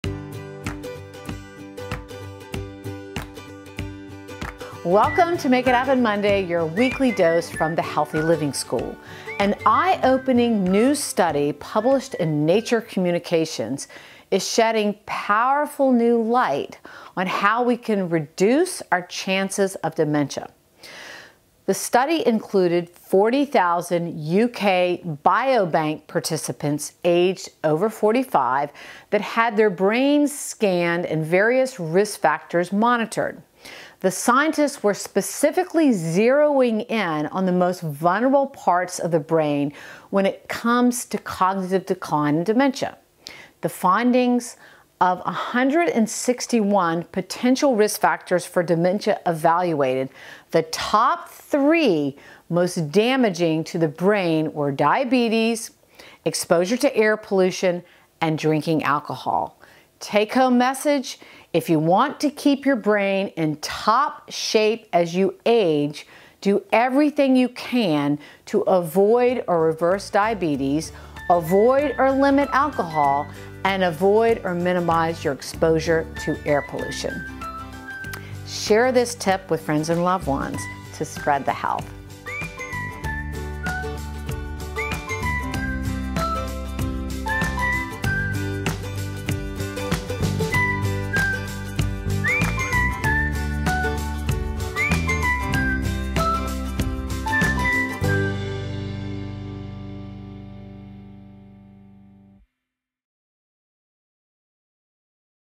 Stay Cognitively Sharp by Avoiding These 3 Things
🚫 An eye-opening new study published in Nature Communications sheds powerful new light on reducing our chances of dementia.

🌟The study included 40,000 UK Biobank participants aged over 45, who had their brains scanned and various risk factors monitored. Scientists zeroed in on the most vulnerable parts of the brain when it comes to cognitive decline and dementia.

🧬🧪The findings? Of 161 potential risk factors for dementia evaluated, the top three most damaging to the brain were diabetes, exposure to air pollution, and drinking alcohol. 🚫🍺💨

Take-home message: if you want to keep your brain in top shape as you age, do everything you can to avoid or reverse diabetes, limit alcohol, and minimize your exposure to air pollution. 🧠✨

Nature Communications, 2024; 15 (1) DOI: 10.1038/s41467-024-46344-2